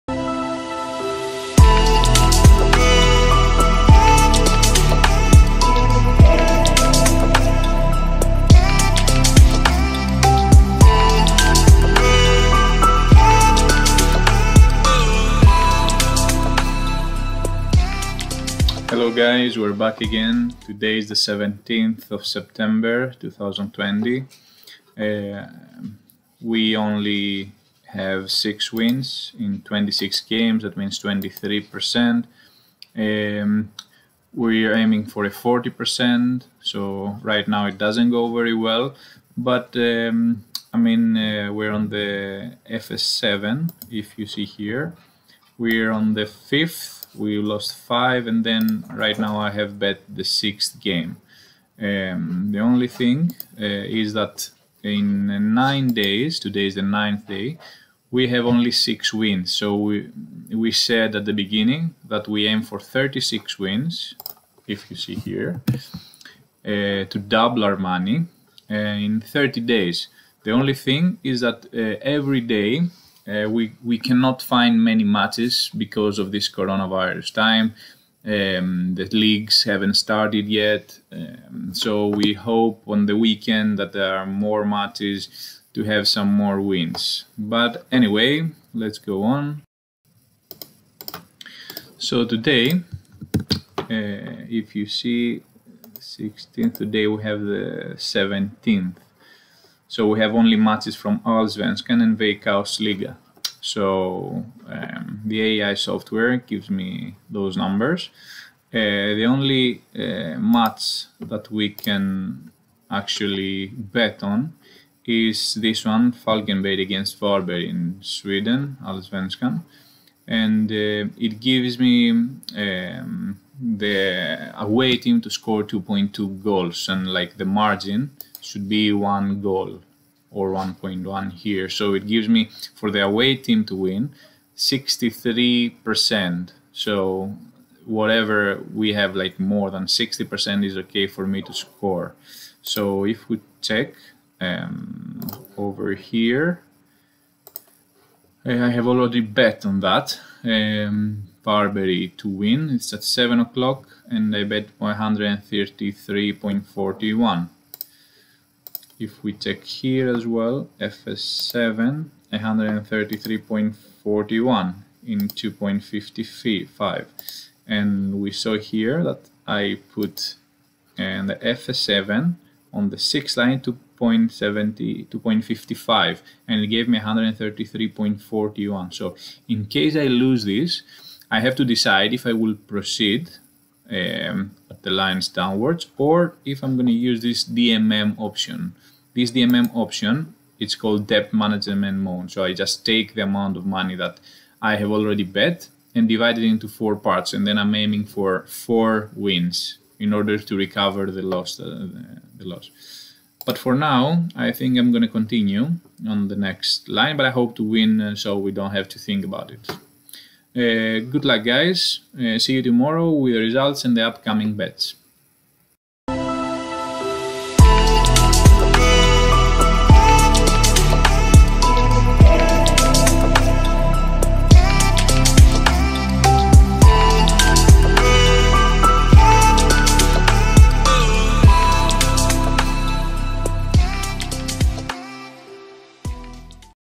Day 9 (17/Sep/2020-Results & Picks) - Machine Learning & AI - Sports Betting
This is the 9th day of our betting journey. I am sharing with you my picks as well as explaining the workflow. Good luck!